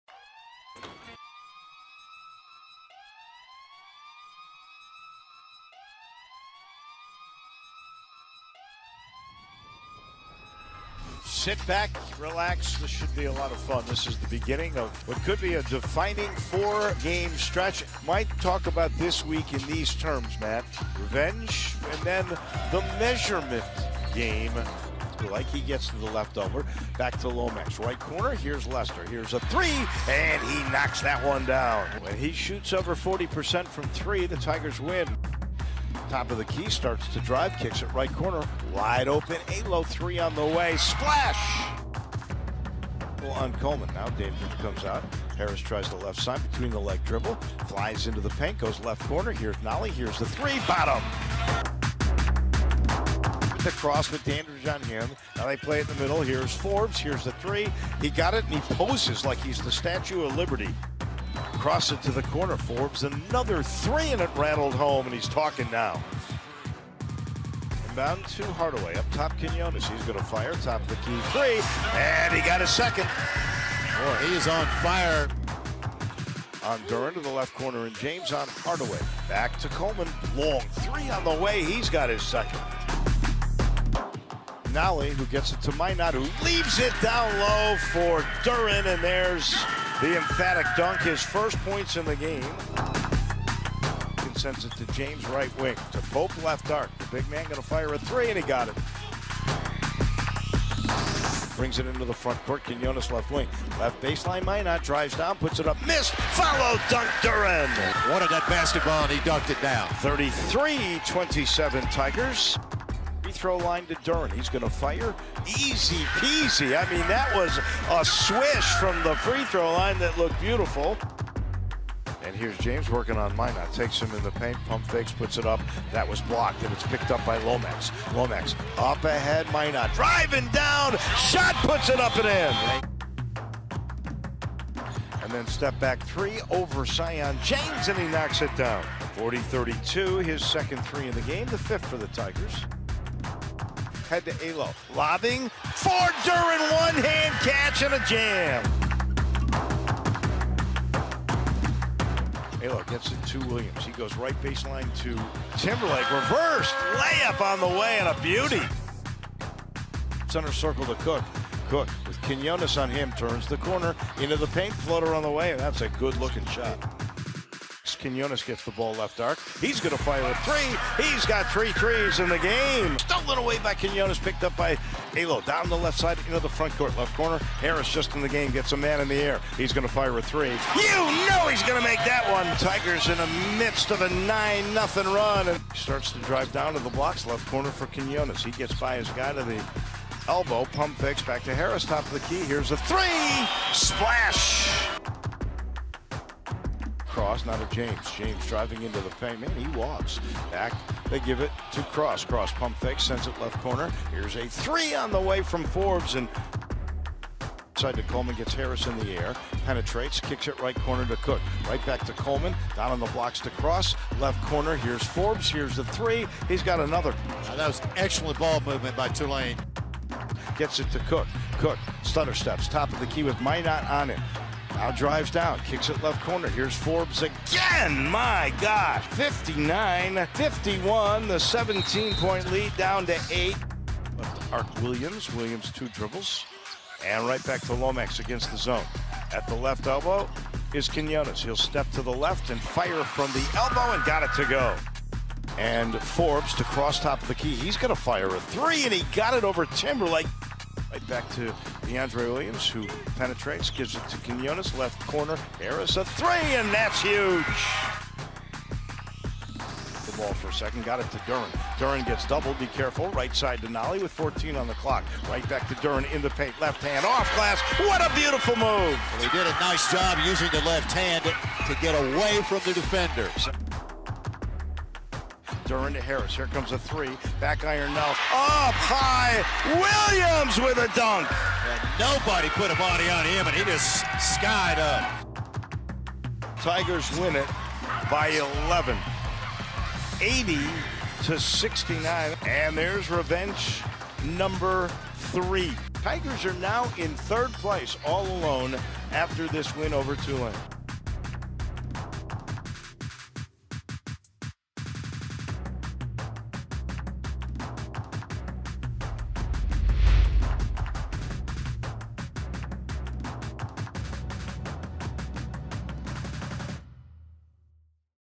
GoTigers247's Mini Film S2E7: Memphis vs Tulane
Check out the sights and sounds from the Tigers' 80-69  revenge win over Tulane on Wednesday, February 9th.

The Tigers were led by Jalen Duren's 15 points and 12 rebounds. Deandre Williams contributed a double double also with 14 points and 13 rebounds.

The Tigers are now 13-8 on the season, and 7-4 in the AAC.